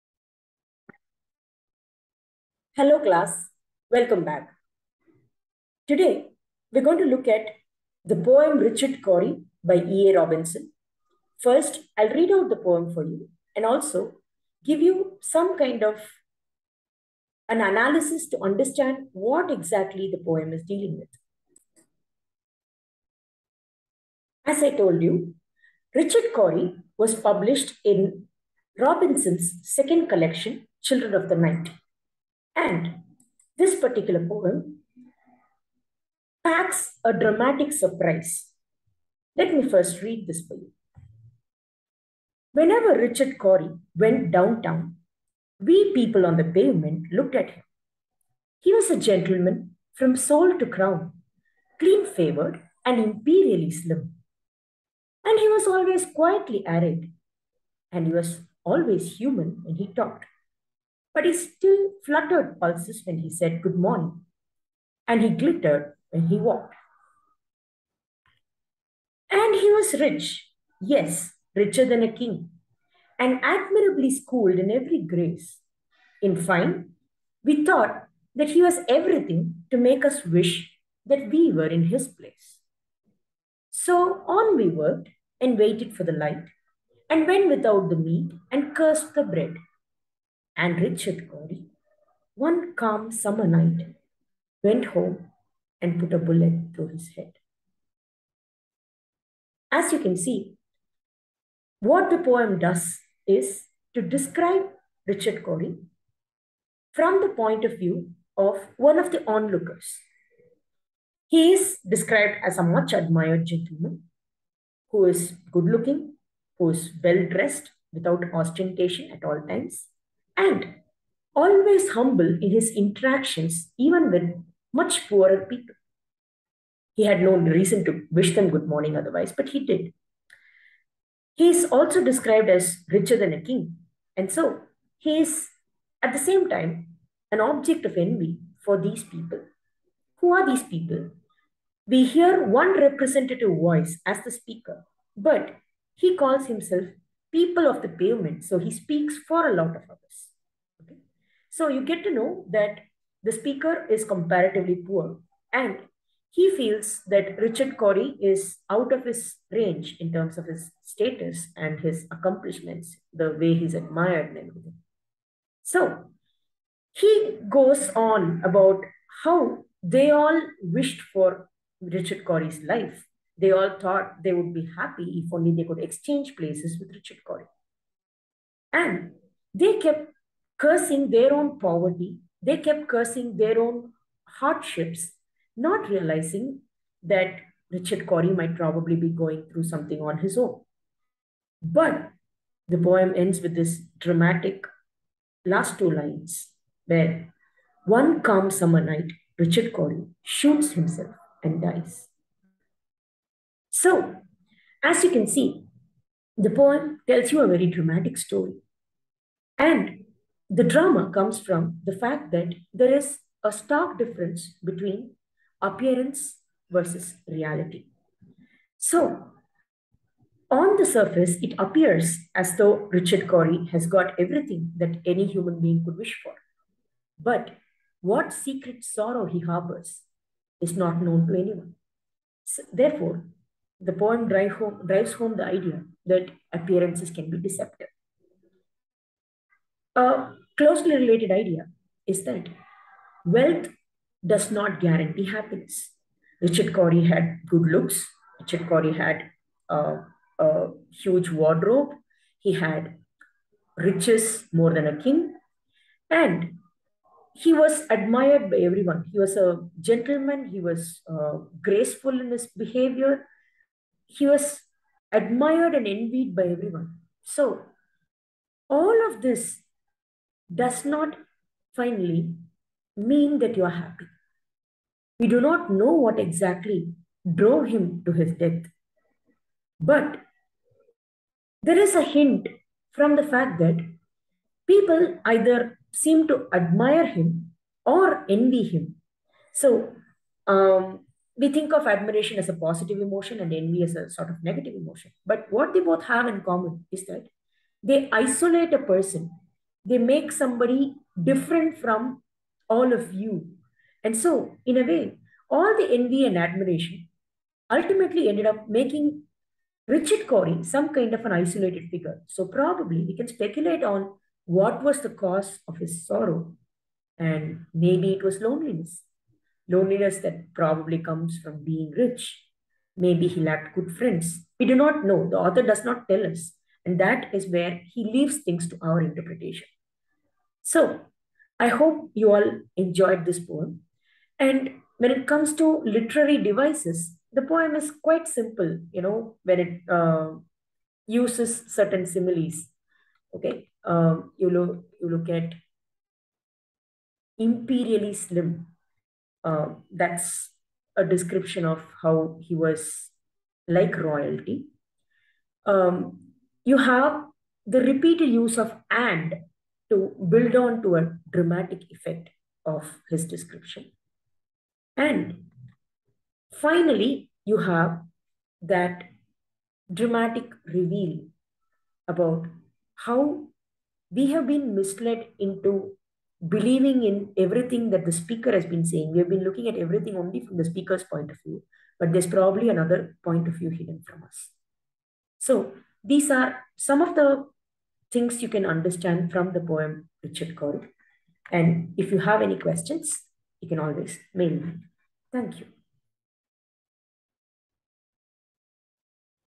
Richard Cory by E.A. Robinson - Reading and Analysis
This video contains a short summary and analysis of his poem "Richard Cory."